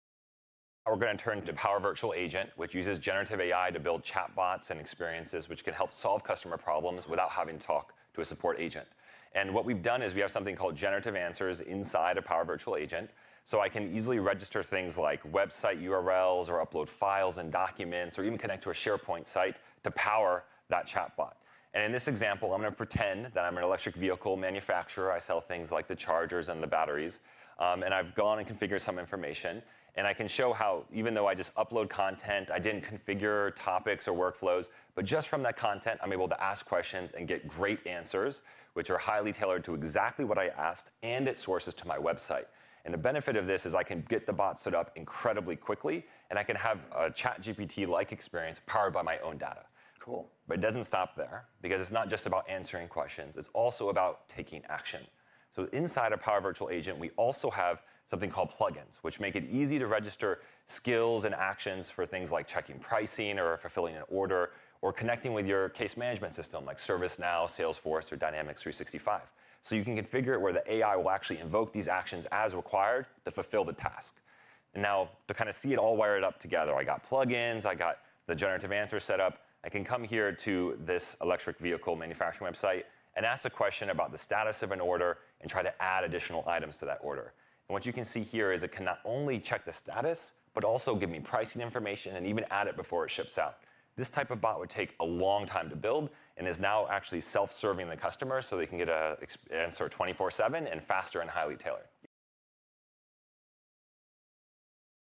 Power Virtual Agents at Microsoft Inspire 2023
At Microsoft Inspire as part of Judson Althoff's keynote, Charles Lamanna, CVP Business Apps and Platforms, shared Power Virtual Agents, an AI-powered solution to build intelligent bots in minutes and respond quickly to customer needs at scale.